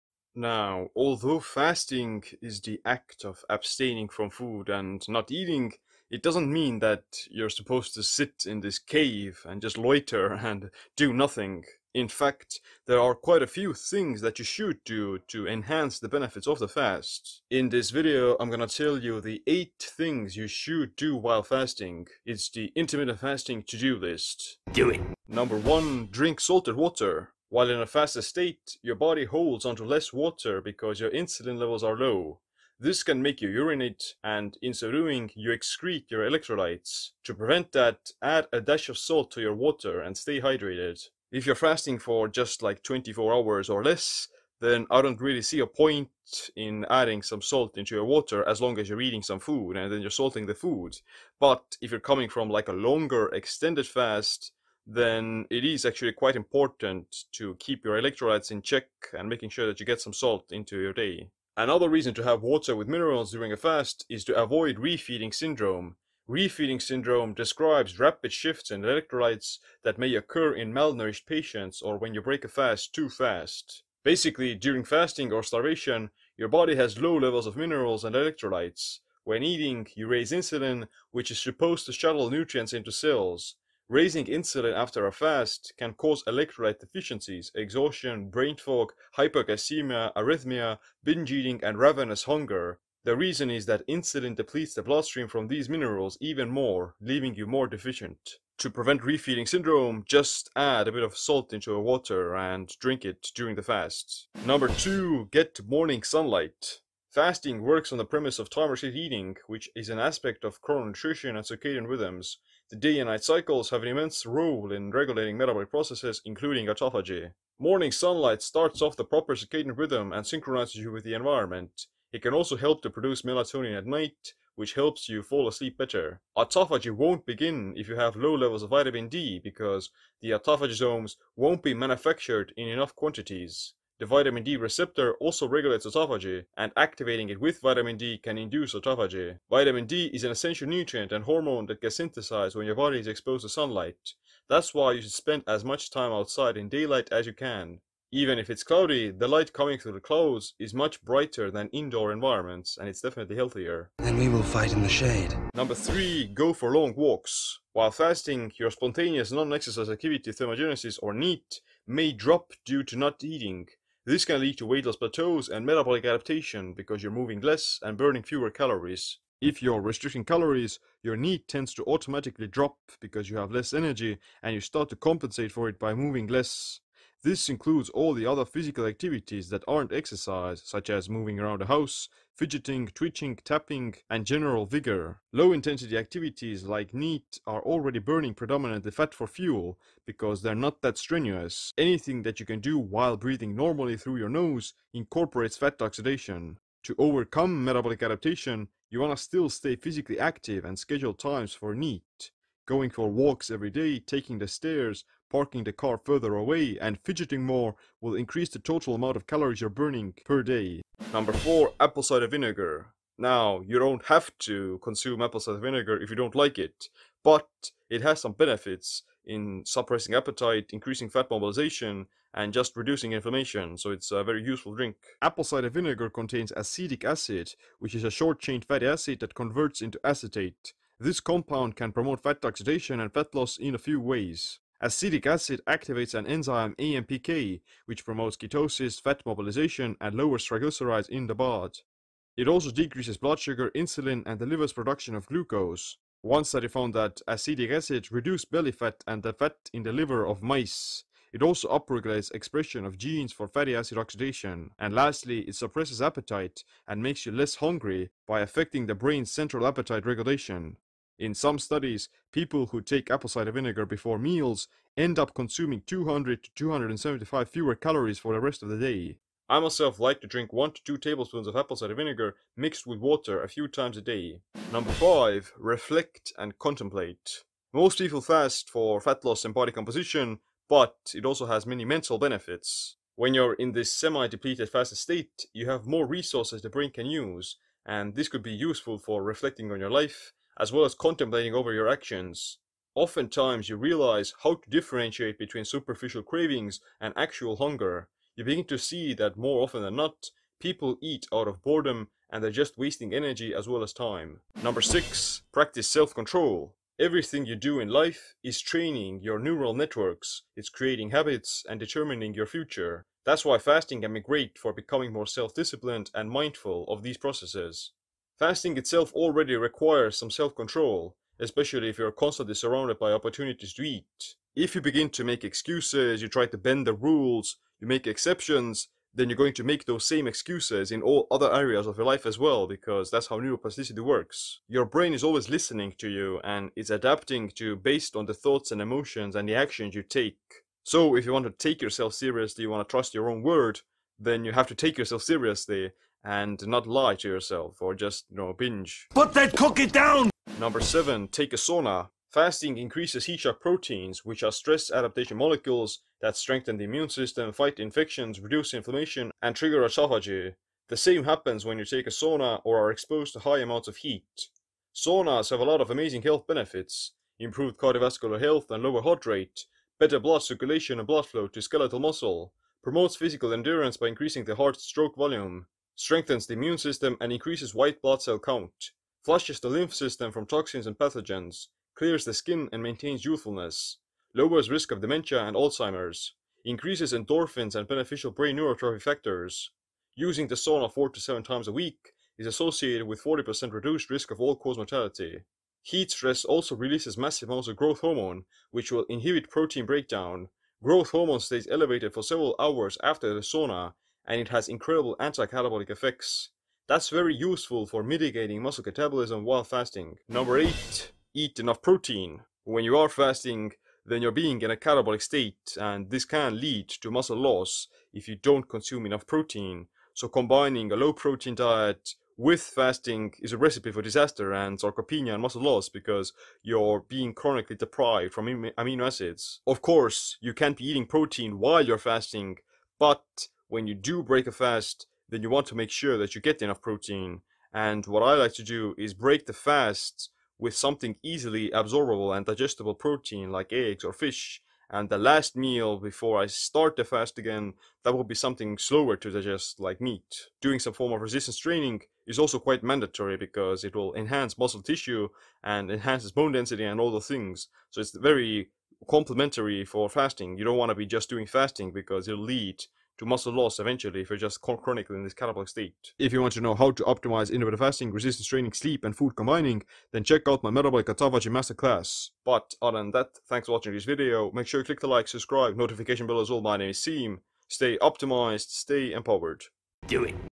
8 Things You Should Do While Fasting - Intermittent Fasting To Do List
Check out the 8 things you should do while fasting.